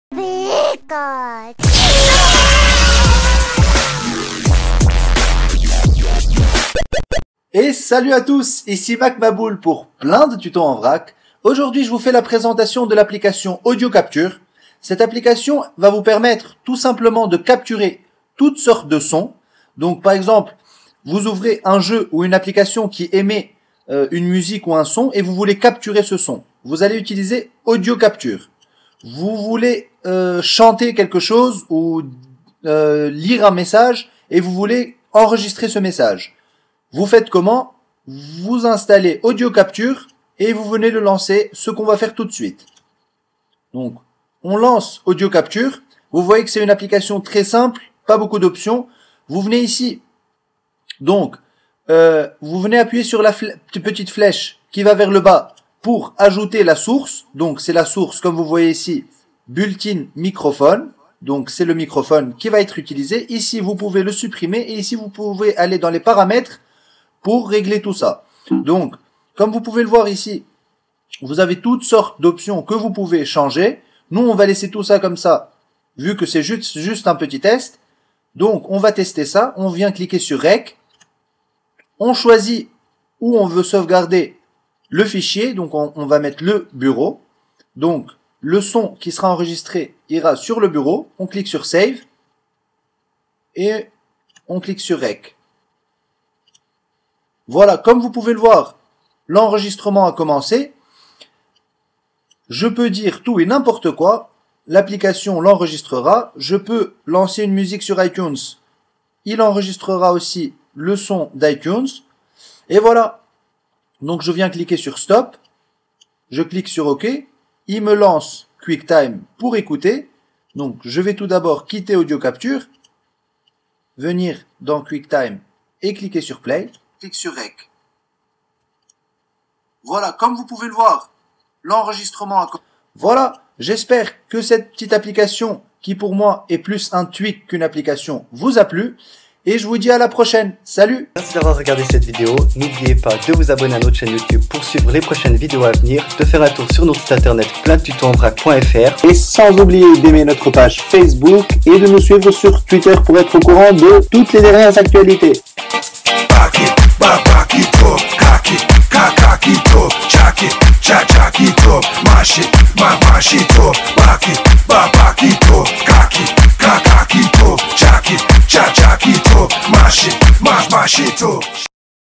Présentation d'AudioCapture: Enregistrer le son de votre micro 
AudioCapture est un utilitaire destiné à gérer et améliorer vos enregistrements audio. Il permet d'enregistrer depuis le micro interne de la machine, l'entrée micro ou une caméra FireWire. Il ne permet pas, par contre, d'enregistrer l'audio de votre Mac.